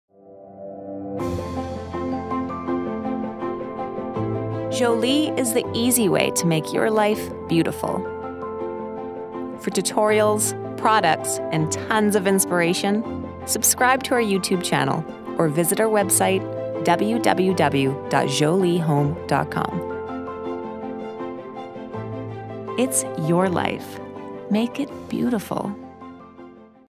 Jolie: the easy way to make life beautiful.
Jolie Home offers premier paints and decorative products for furniture, floors, walls, and accessories. It's the easy way to transform your home.